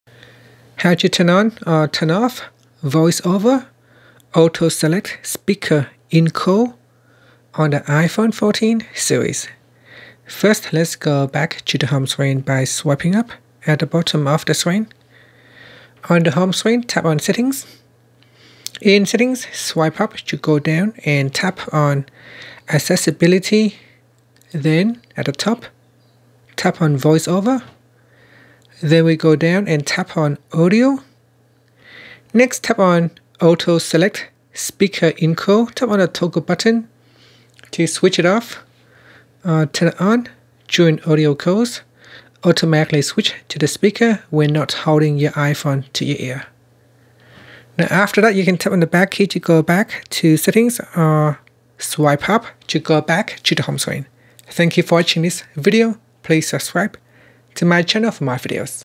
iPhone 14's/14 Pro Max: How to Turn On/Off VoiceOver Auto-Select Speaker In Call
Learn how you can turn on or off VoiceOver auto-select speaker in call on the iPhone 14/14 Pro/14 Pro Max/Plus.